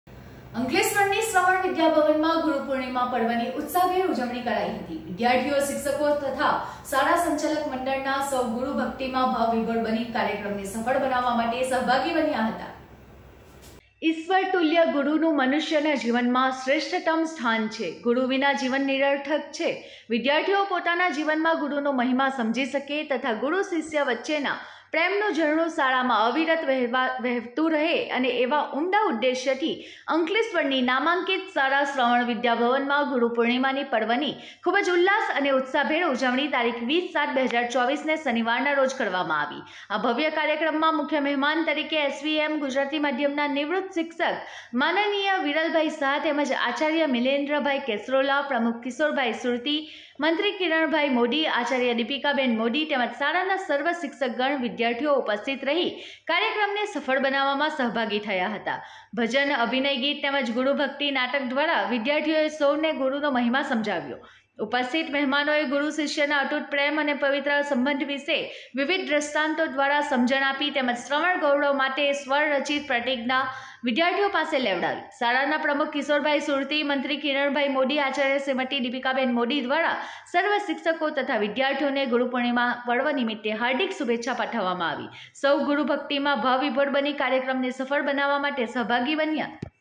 માઁ ટીવી સમાચાર 20.07.2024અંકલેશ્વર ની શ્રવણ વિદ્યાભવનમાં ગુરુપૂર્ણિમા પર્વની ઉત્સાહભેર ઉજવણી
માઁ ટીવી સમાચાર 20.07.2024ગુરુપૂર્ણિમા : અંકલેશ્વર ની શ્રવણ વિદ્યાભવનમાં ગુરુપૂર્ણિમા પર્વની  ઉત્સાહભેર ઉજવણી

વિદ્યાર્થીઓ, શિક્ષકો સહિત શાળા સંચાલક મંડળ ના સૌ ગુરુ ભક્તિમાં ભાવવિભોર બની કાર્યક્રમને સફળ બનાવવા માટે સહભાગી બન્યા.